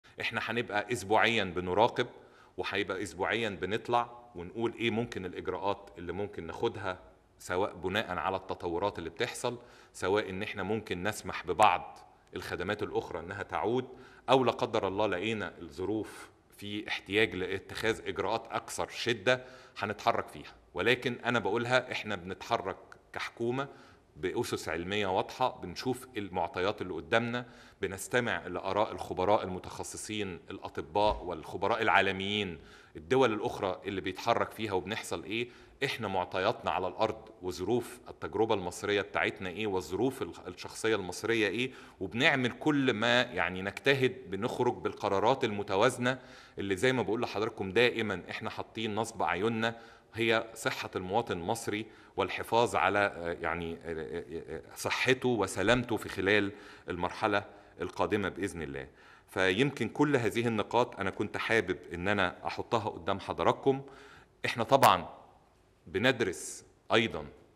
الآن| رئيس الوزراء: ندرس إعادة بعض القطاعات الأخرى للعمل وخاصة السياحة الداخلية
|   23إبريل 2020